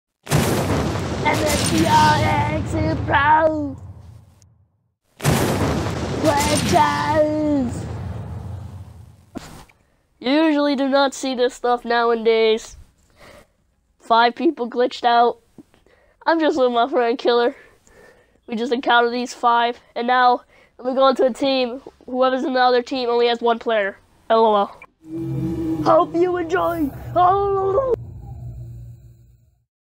MSTR x SUPERPRO glitches part 1 (me, killer, and our team vs just 1 player)
Hope u enjoyed this amazing glitch!